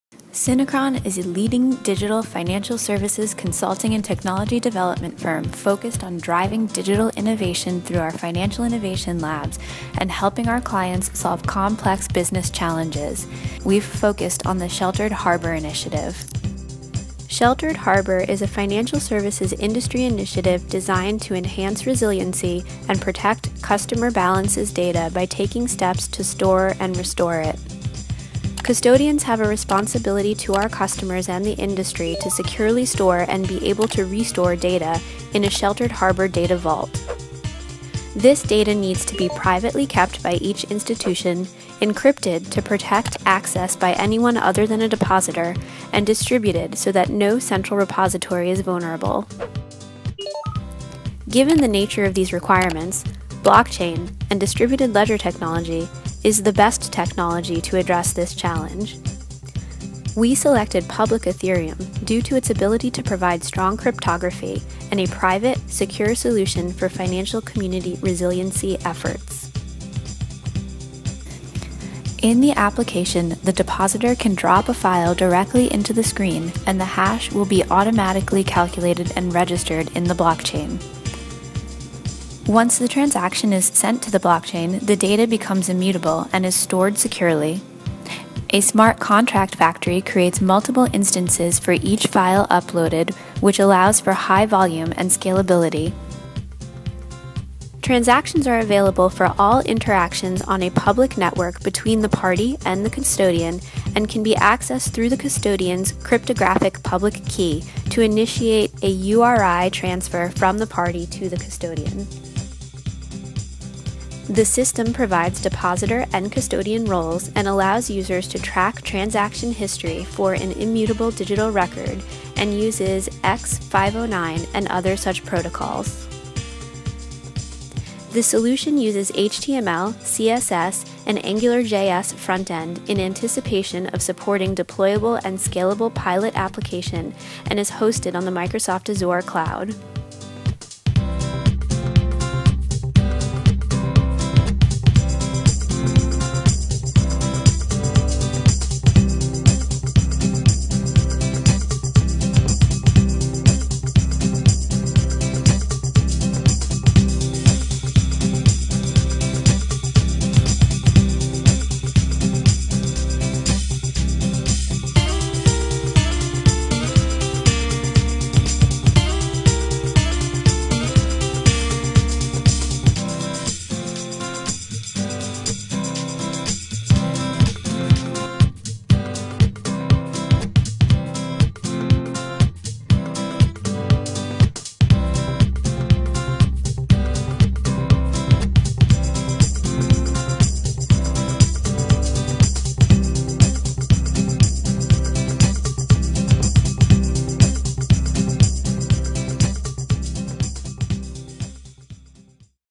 Blockchain POC Takes Disaster Recovery/Resiliency to the Next Level
Disaster recovery and resiliency are major challenges for the financial services industry with many new, innovative initiatives being explored on how to address this across peer-to-peer networks. Synechron was asked by a large custodian bank to analyze existing and evolving standards and to develop a decentralized application (DApp) that would build on them to enhance resiliency and protect customer information. The resulting Proof of Concept (POC) proved successful in proving that by using a distributed network to implement this standard, client data will be more secure.